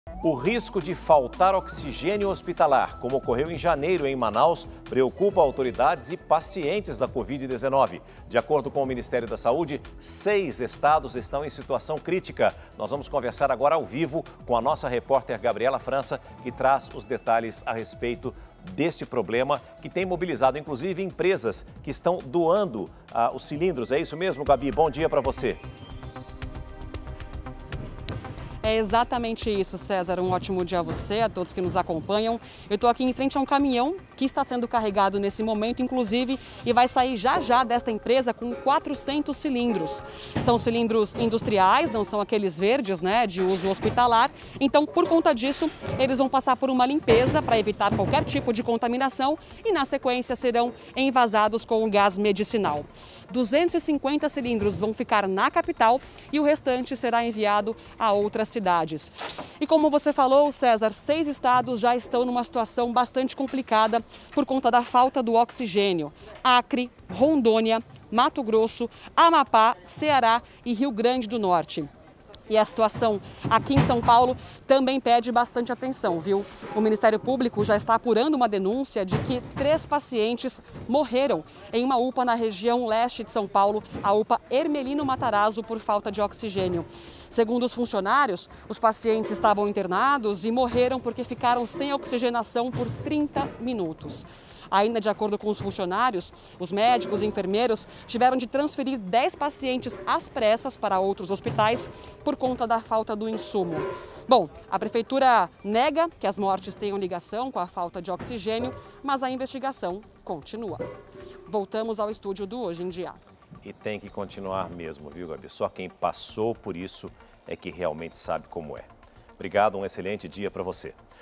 Indústrias doam 400 cilindros de oxigênio para tratar pacientes com covid-19 em SP
O risco de faltar oxigênio hospitalar, como ocorreu em janeiro em Manaus, preocupa autoridades e pacientes da covid-19. De acordo com o Ministério da Saúde, seis estados estão em situação crítica.